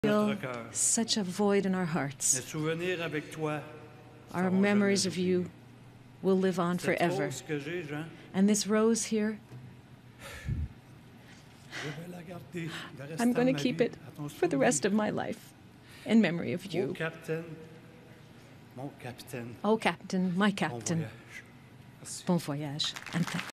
Yvan Cournoyer Eulogy clip for Jean Beliveau
Even the interperator has a hard time keeping it together.

Jean, we feel great emptiness in our hearts. My memories with you will never be forgotten.  This rose I have, Jean, I will keep it the rest of my life, in your memory.  Oh captain, my captain. Bon voyage.